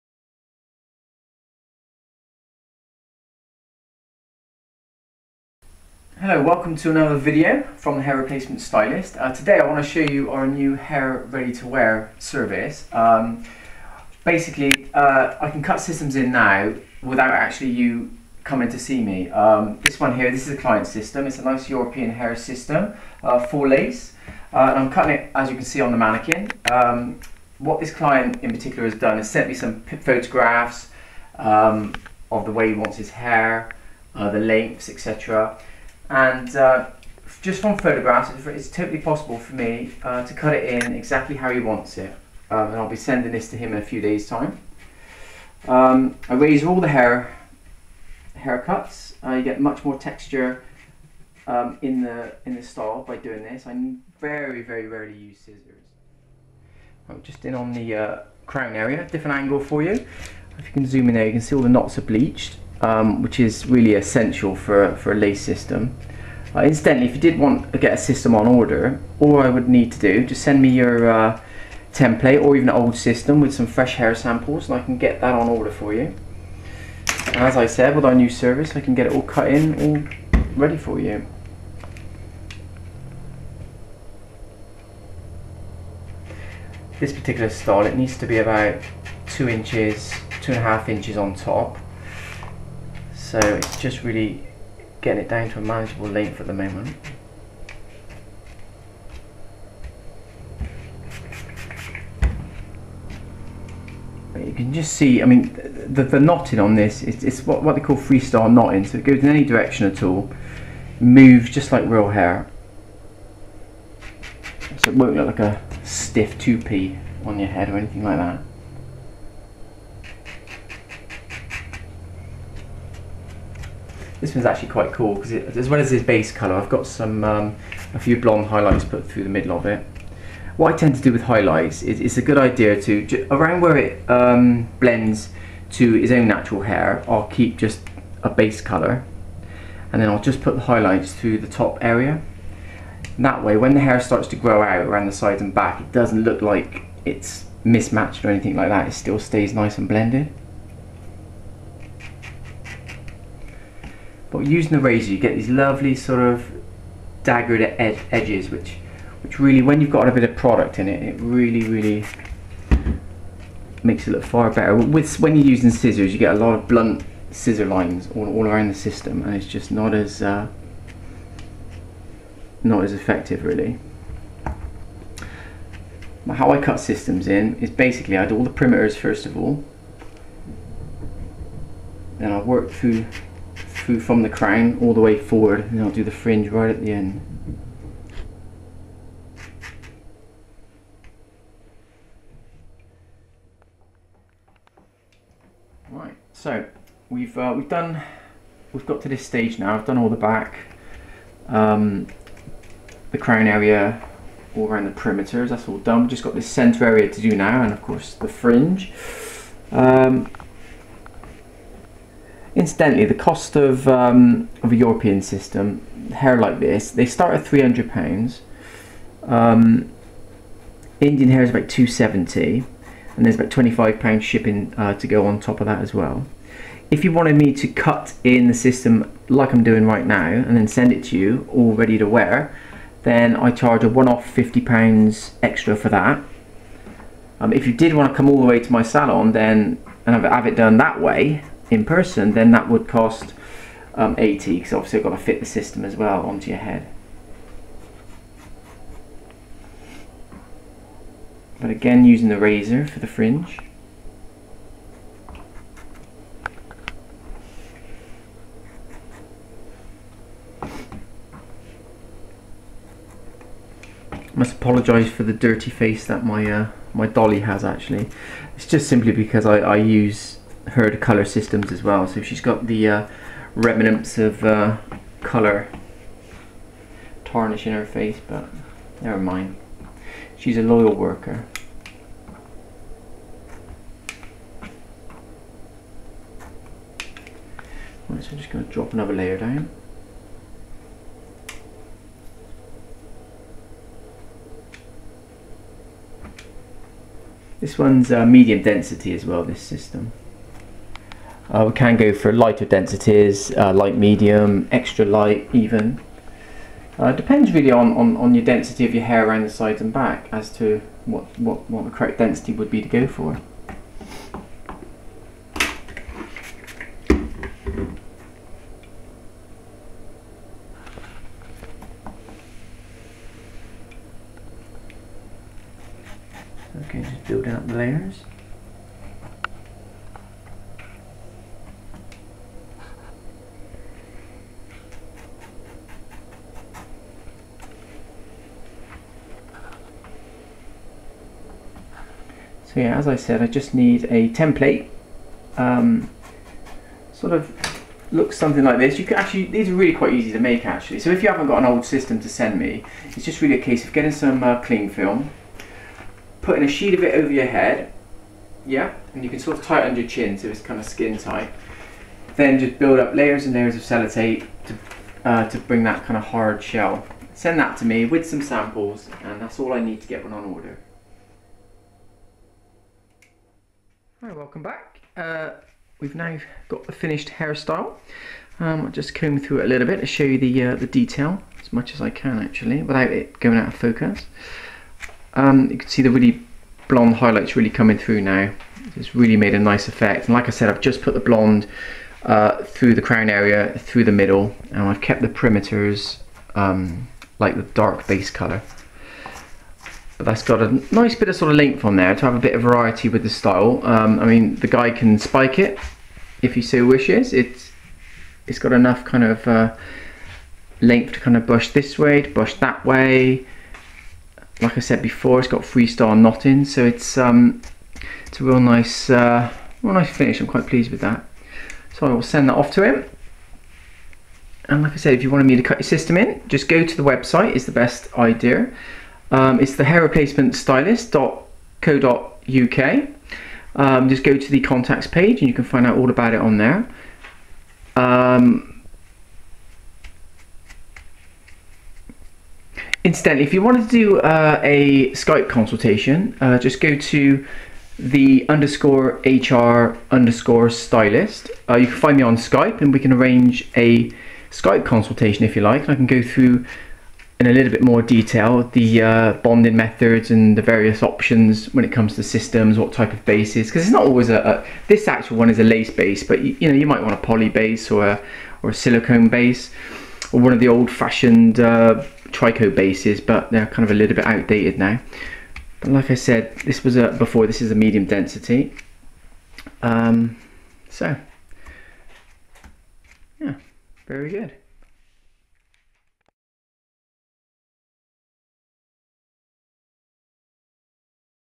Hair Replacement System, Hair System cut in
Introducing our new hair ready to wear service from the I can cut and style any hair system from photos you send me. This video demonstrates me cutting and blending a clients new European Hair System.